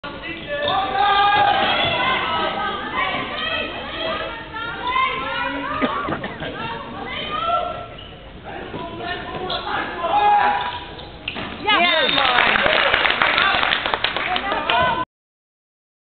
Sporting Delta A2 - KCR A2
Nathan was goed op dreef in alle zaken die korfbal zo leuk maken waaronder scoren